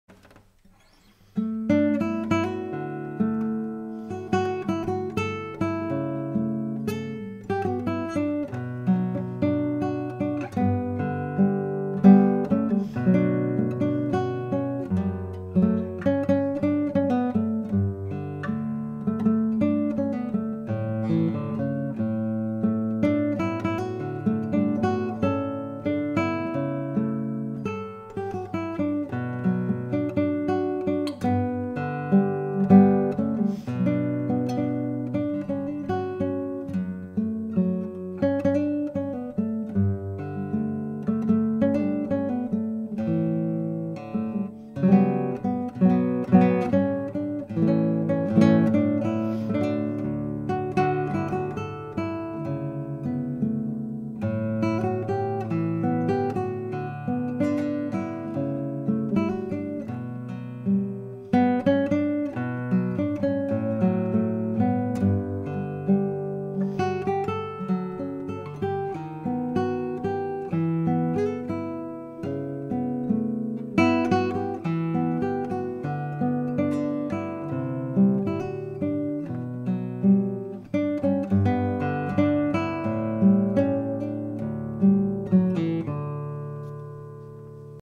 2003 David Rouse classical guitar
Here is a 2003 David Rouse classical guitar.  Rouse is a veteran British luthier building traditional guitars. Information about this and comparable instruments can be found at Chartwell Guitar, or chartwellguitar.com.